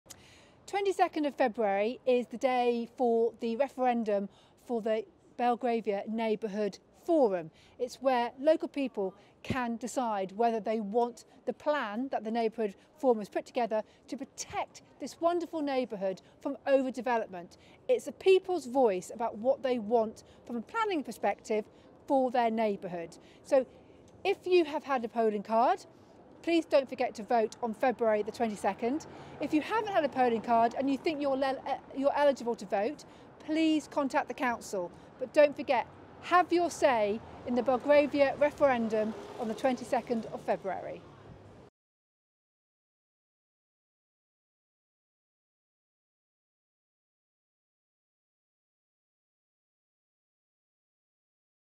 12 Feb - Have your say on the Belgravia Neighbourhood Plan
Cities of London & Westminster MP, Nickie Aiken, is encouraging everyone who lives in Belgravia to have their say and vote in the Belgravia Neighbourhood Plan referendum on Thursday, 22 February.

Nickie commented,

"22 February is the day for the referendum for the Belgravia Neighbourhood Forum. It's where local people can decide whether they want the plan that the Neighbourhood Forum has put together to protect this wonderful neighbourhood from overdevelopment. It's a people's voice about what they want from a planning perspective for their neighbourhood. So if you have had a polling card, please don't forget to vote on 22nd February."

If you haven't had a polling card and think you're eligible to vote, please get in touch directly with Westminster City Council.